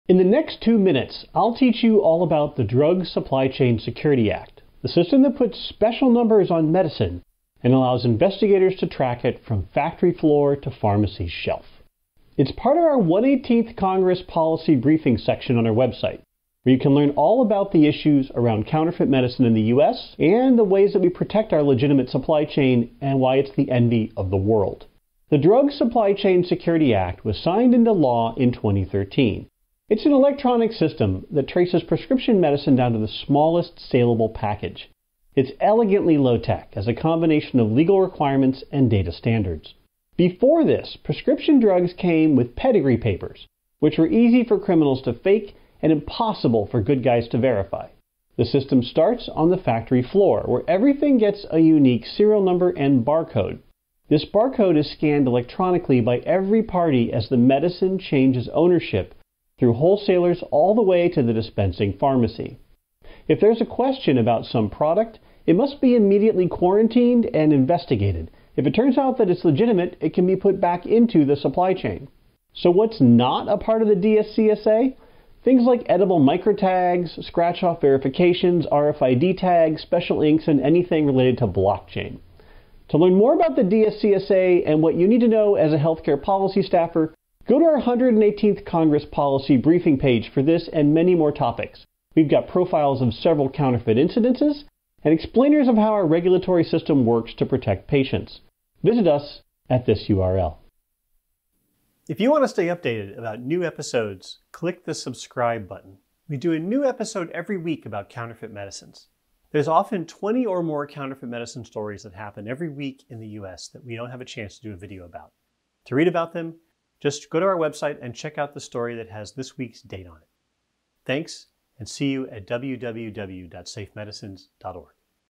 Track and Trace in 90 Seconds
Learn all about the Drug Supply Chain Security Act, the system that puts serial numbers on medicine and allows investigators to track it from factory floor to pharmacy shelf. It’s part of our 118th Congress policy briefing section on our website, where you can learn all about the issues around counterfeit medicine in the U.S. and why our legitimate supply chain is the envy of the world